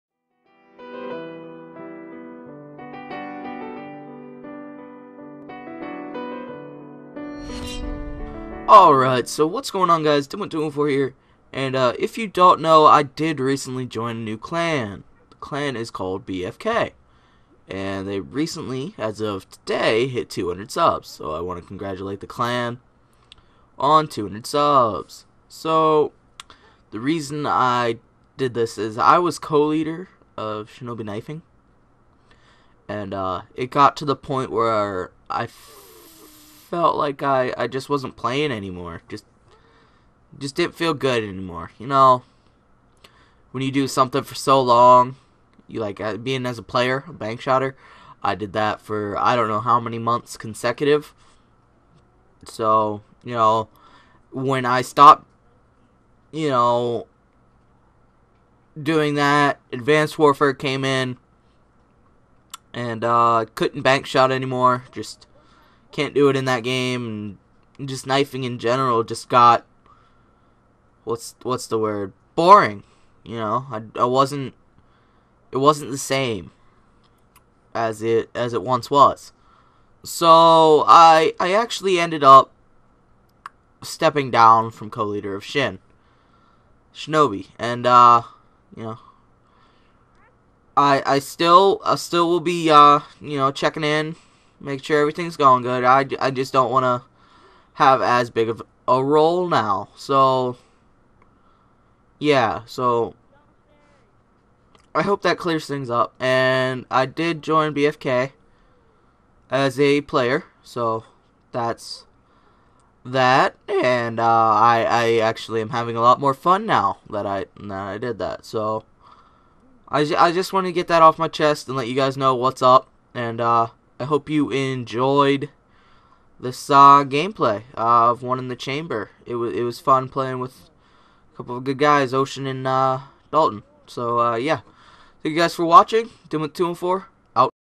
Why i joined BFK | Let's Talk [#1]
Please guys don't be mad that I've decided to step down I hope this clears some things up.